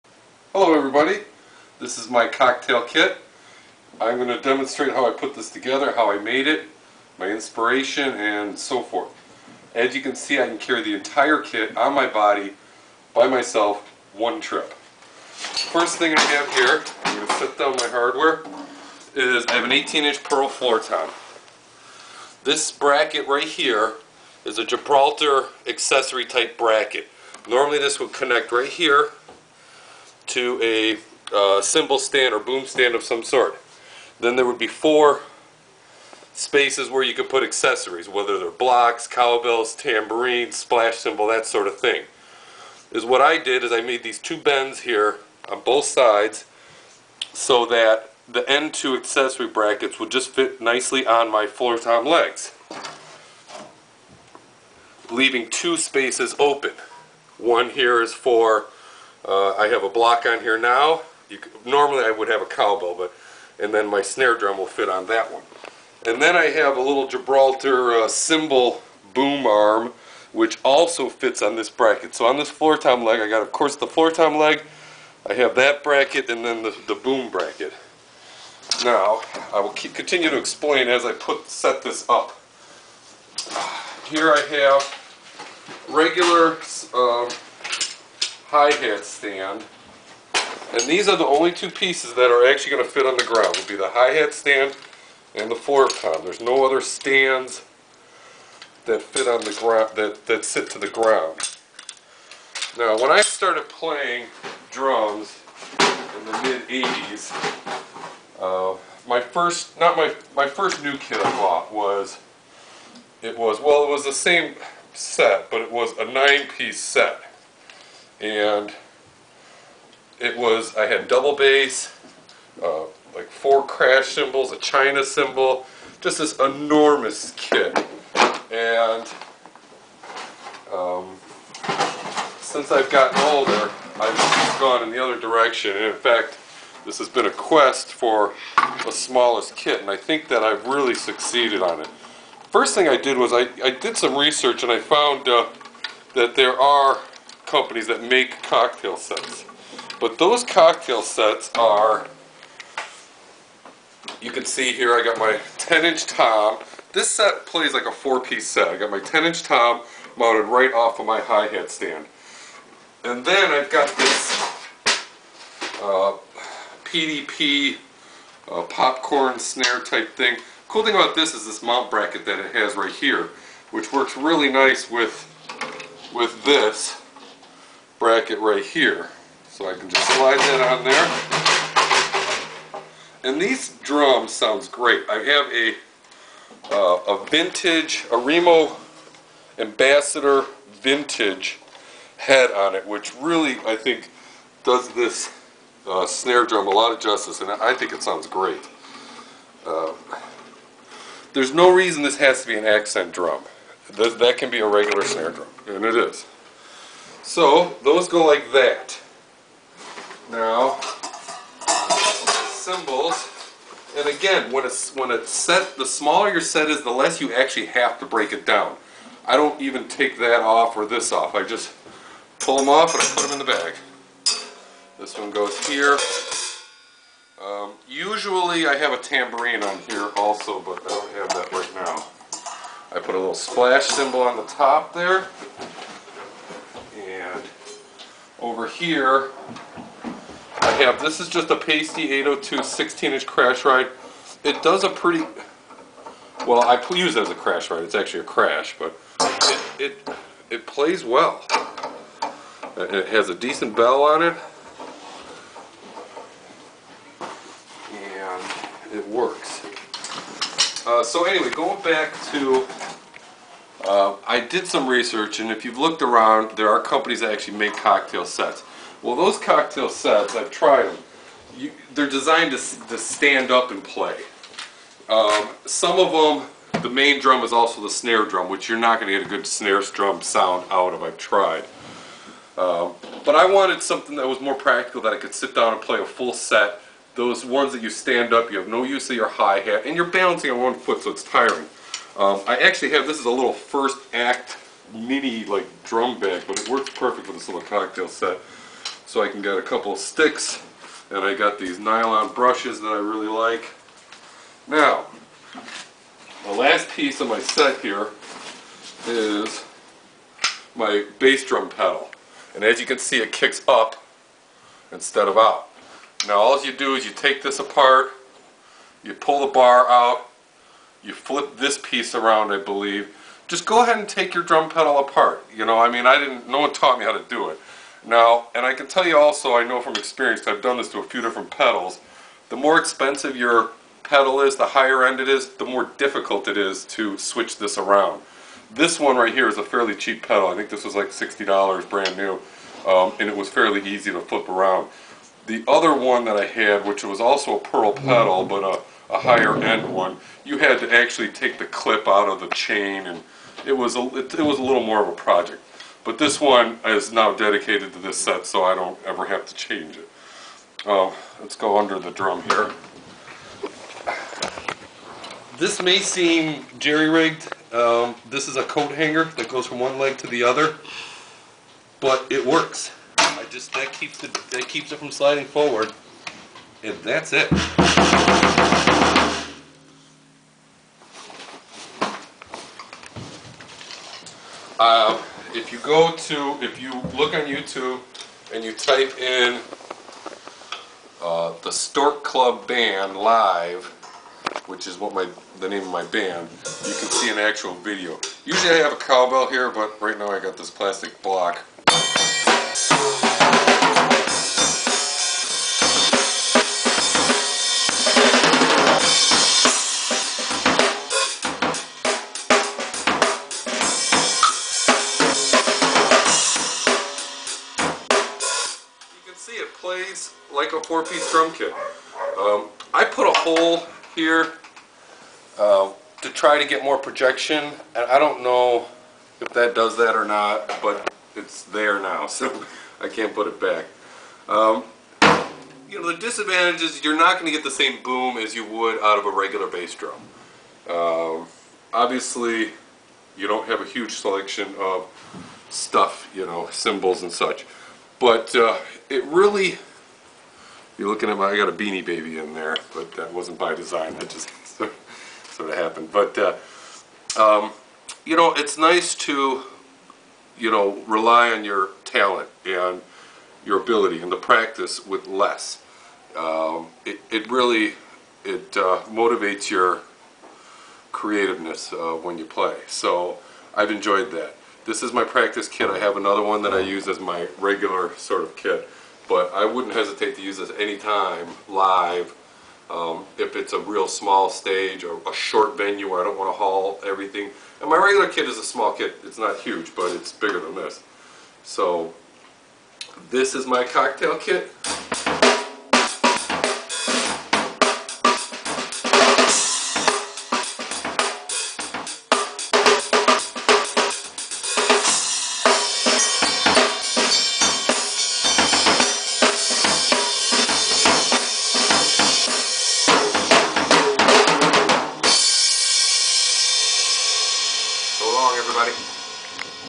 Cocktail drum kit & how to make it
My mini portable drum kit with demonstration, with popcorn snare, and floor tom bass drum. this plays like a four piece set. Worlds smallest drum kit. Trap kit.  How to. Easy drum kit breakdown!